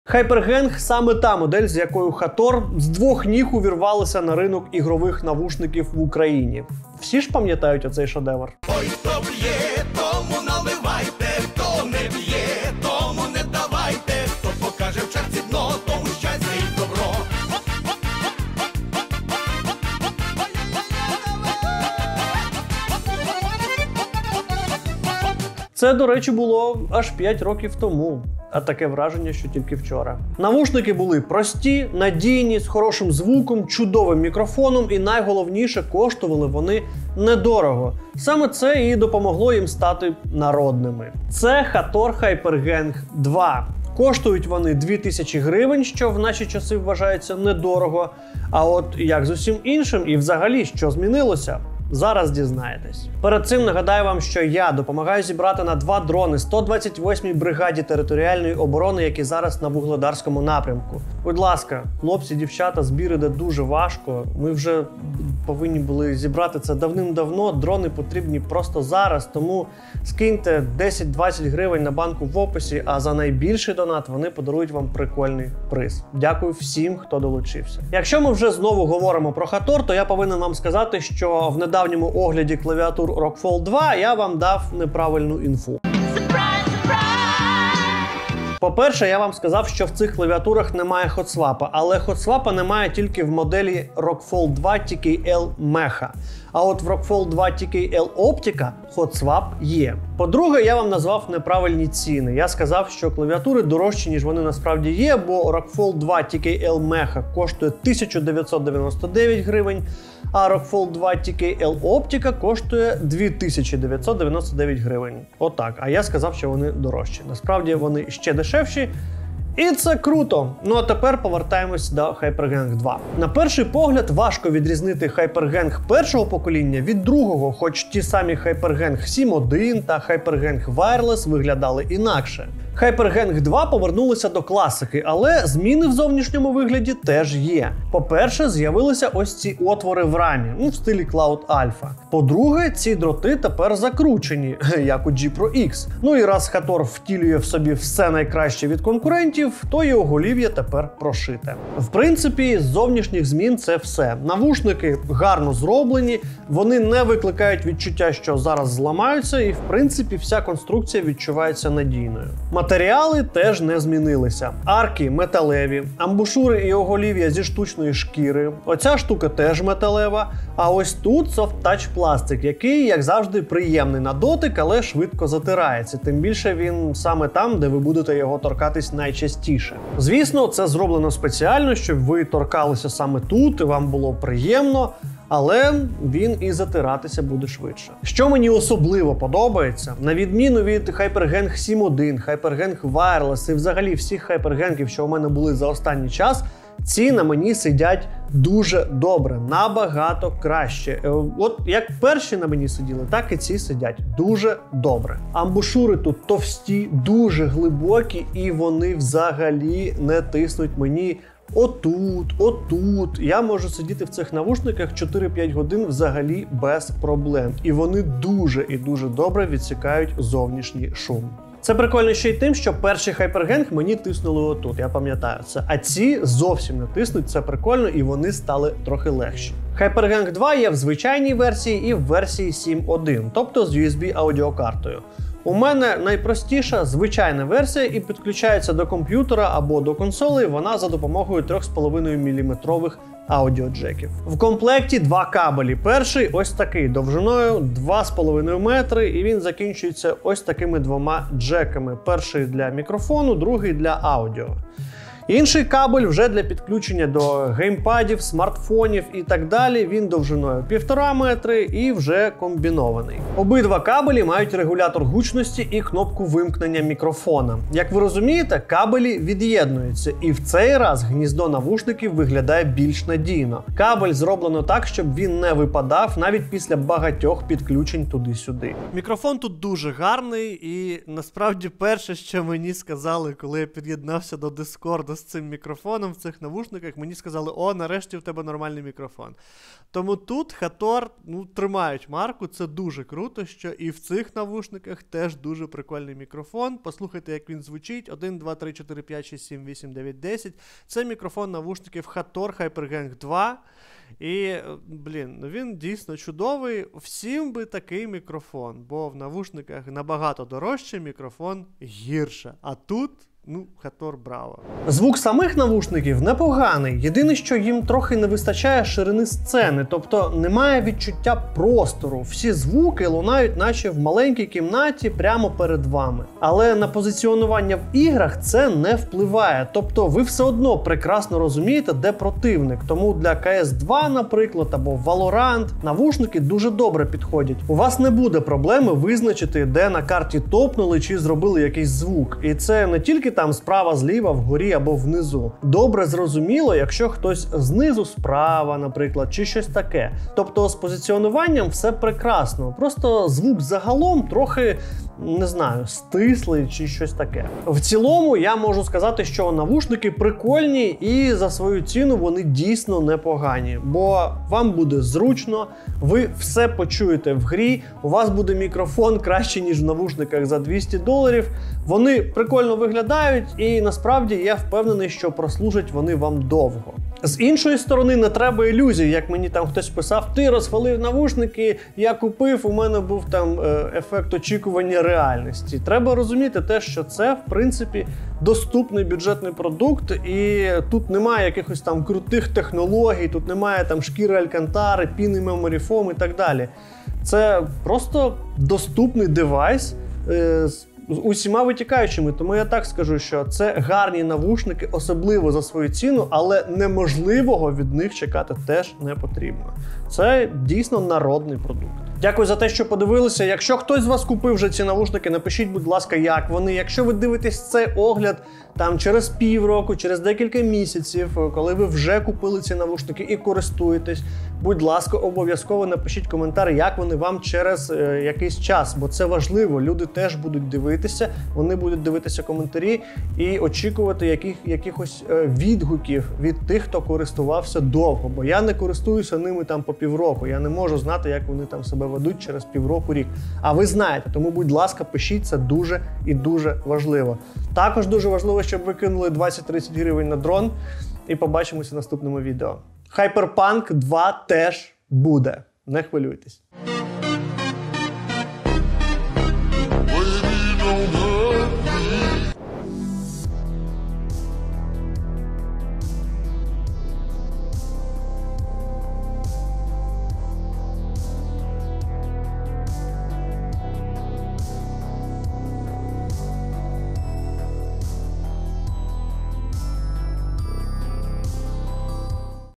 Hator Hypergang 2 - НАЙКРАЩИЙ МІКРОФОН в НАВУШНИКАХ!
Hator HYPERGANG 2 - навушники, які дуже добре наслідують першу версію. Тільки тут все краще - вони легше, тут більш зручні кабелі і вони трохи покращили дизайн. Все інше - як і було: чудовий мікрофон, надійна конструкція і непоганий звук.

Підтримати канал:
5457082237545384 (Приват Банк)

🤟🤟🤟 Приєднуйся до моєї партнерської мережі і знімай годноту!